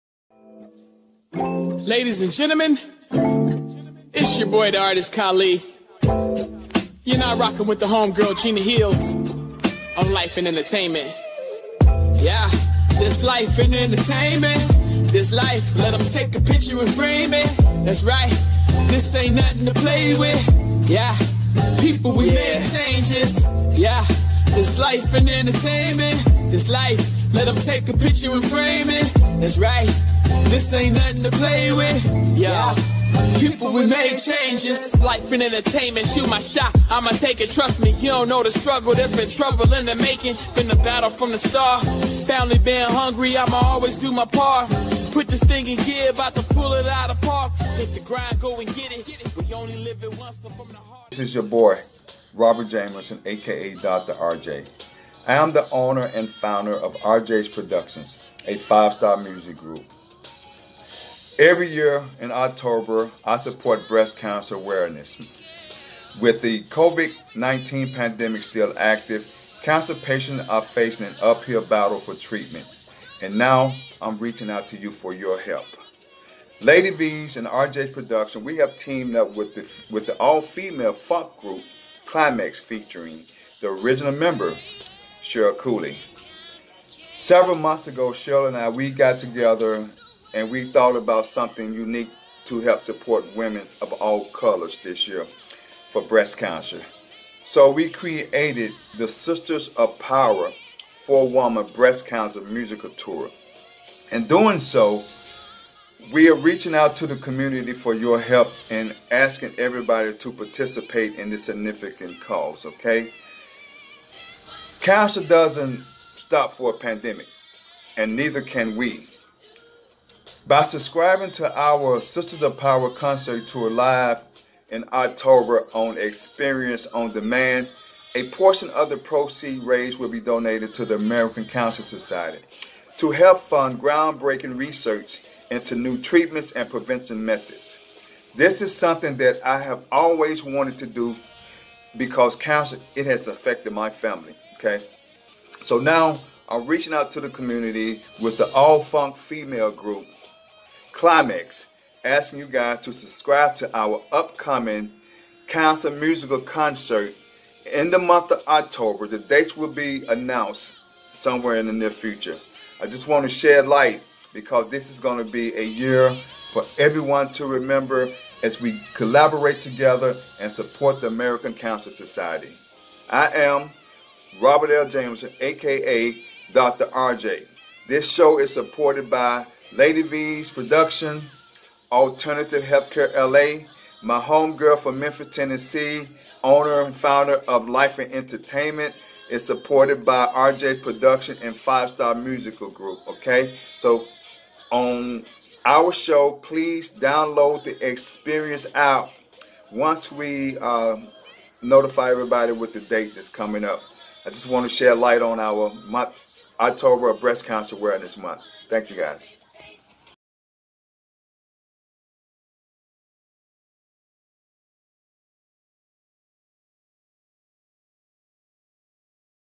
Rjai's Productions & 5 Stars Music Group Presents The Sisters of Power 4 Woman Breast Cancer Musical
Rjai's and Ladivee's Production 8th Annual Breast Cancer Musical Concert A Sisters  of Power "4 Woman" Breast Cancer Musical Tour a 5 Stars Music Group Concert Series with KLYMAXX Featuring Legendary Cheryl Cooley Sponsored by Gena Heelz C.E.O. Life In Entertainment. Supported by Alternative Healthcare LA The American Cancer Society Verdiana Bentley-Brodie Ladivee's Production and Entertainment